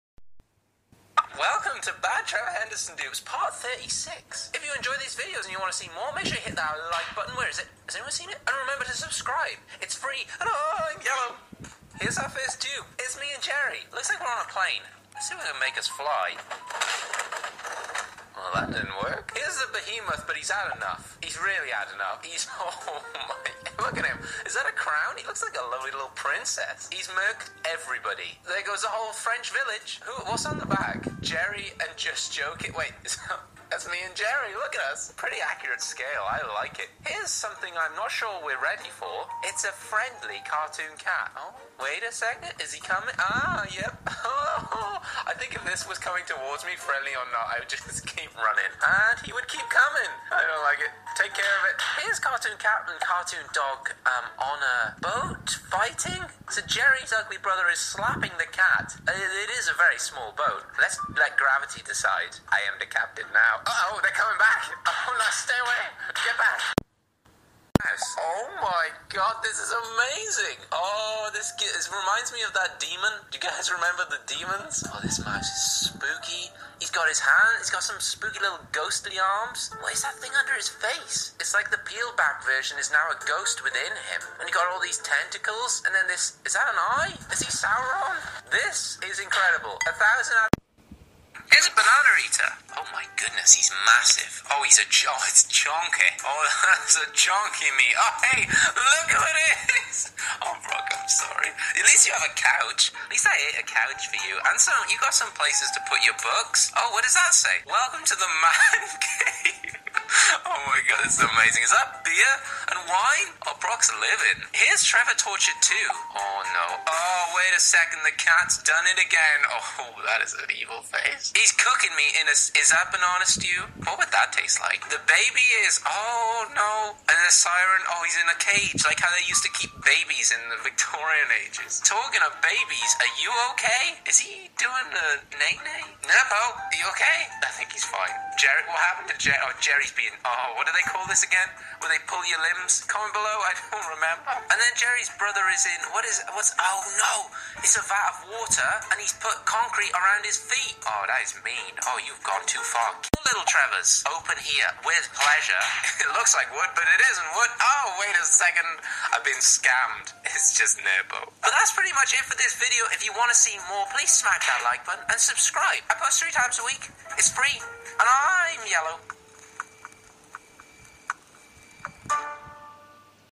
BAD TREVOR HENDERSON DUPE PART 36 BUT I ANIMATED
I FORGOT TO INTRODUCE MYSELF
REMEMBER THE LAST VIDEO I FORGOT TO ADD DESCRIPTIONS BTW I'M 11 AND I LIKE TO DRAW
APP I USE:
XRECORDER TO GET THE AUDIO
FLIPACLIP TO DRAW
KINEMASTER (YEAH I USE KINEMASTER CUZ ALIGHT MOTION IS LAGGING WHEN USING AUDIO I BEEN EDIT LAST NIGHT HOPEFULLY IT'S WORTH IT)